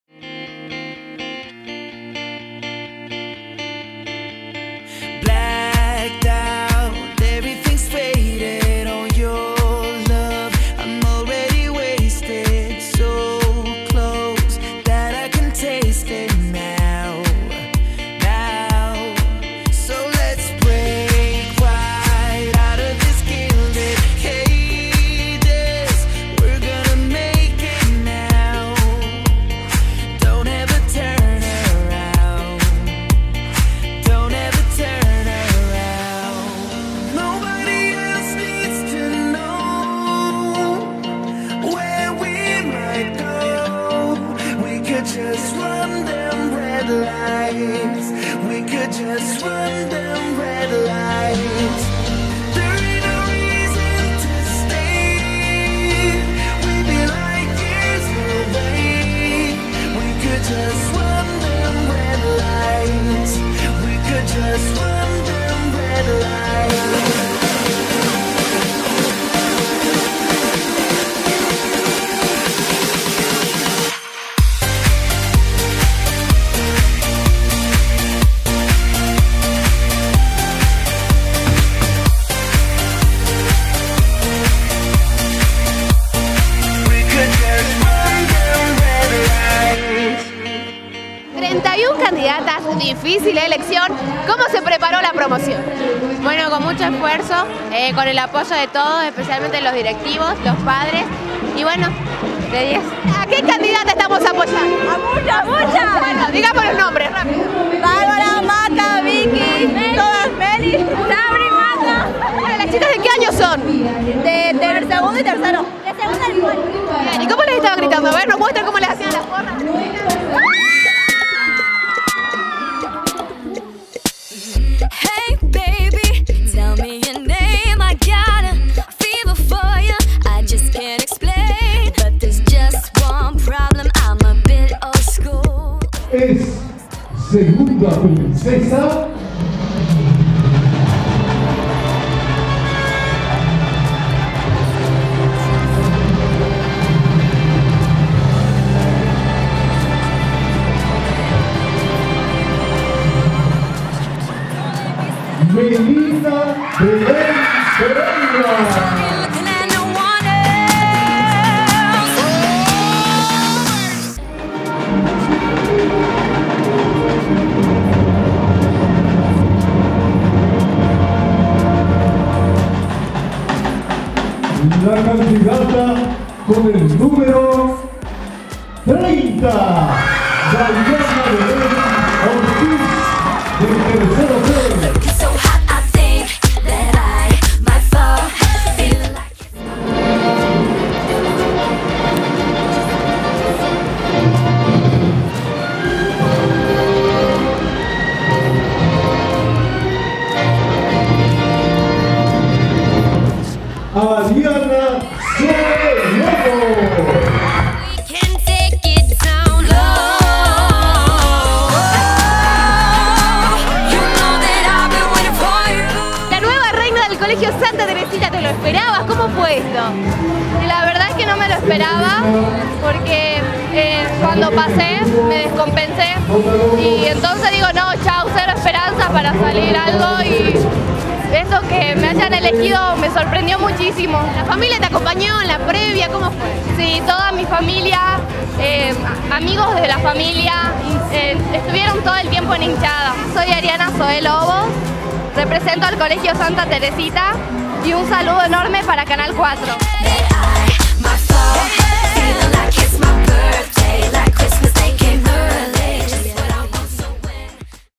Ariana fue la más linda en el Santa Teresita
El Colegio Santa Teresita sumó un nuevo nombre para lo que será la elección Reina Capital en el mes de septiembre. La joven Ariana Lobos fue la elegida entre más de treinta candidatas al trono.